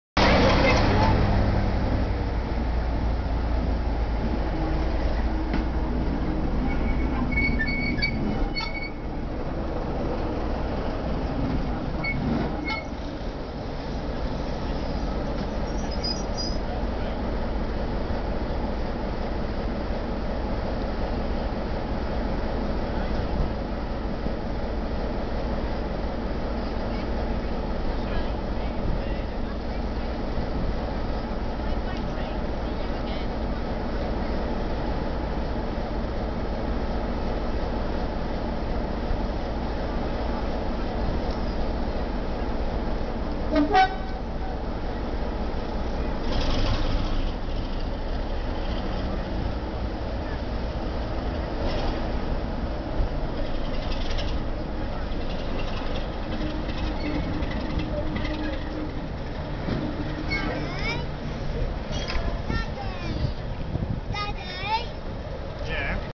Spa Valley Railway 15
After arriving at Tunbridge Wells West with the 1530 from Eridge, the Spa Valley Railway's resident cl.33 diesel no.33063 "R.J.Mitchell" runs round its train prior to forming the 1620 back to Eridge.

The building in front of the loco is the original Tunbridge Wells West station, now a restaurant.